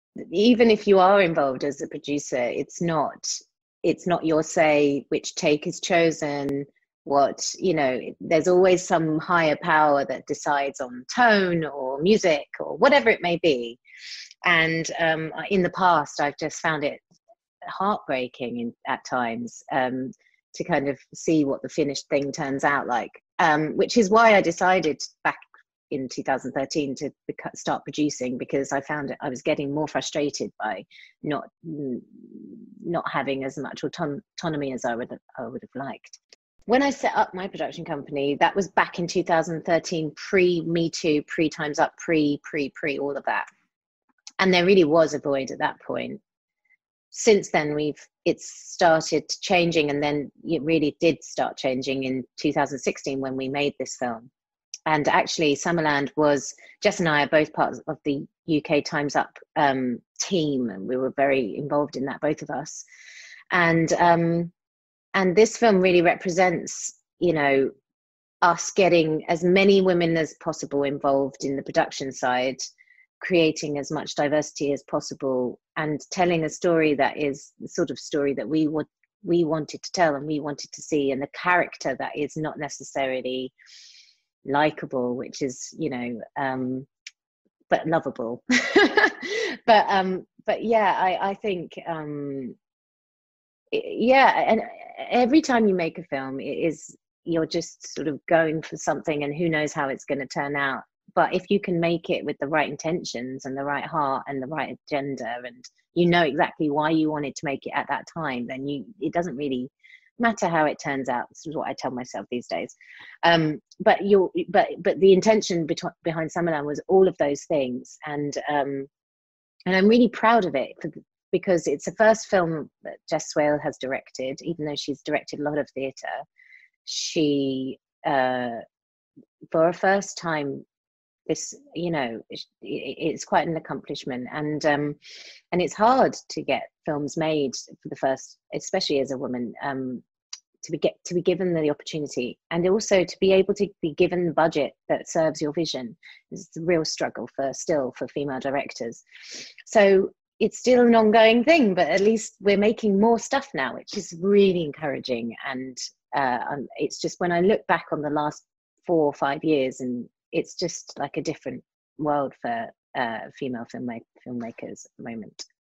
Gemma Arterton Has Producer Status, But Still Relies On Trust
After achieving success in films like Quantum of Solace and Clash of the Titans, British actress Gemma Arterton has used her voice on behalf of women's right in the movie industry, and she's put her money where her mouth is: Since 2016, she's run a production company that specializes in projects run by and starring women. The company's projects include Summerland, a World War II-era drama directed by Jessica Swale. Arterton stars in the film and is also its executive producer, but even with that much involvement in the film, she concedes that the final cut is out of her hands, and she's come to peace with that, especially having handed the reigns over to Swale..

Summerland is now available for streaming rentals on most digital platforms.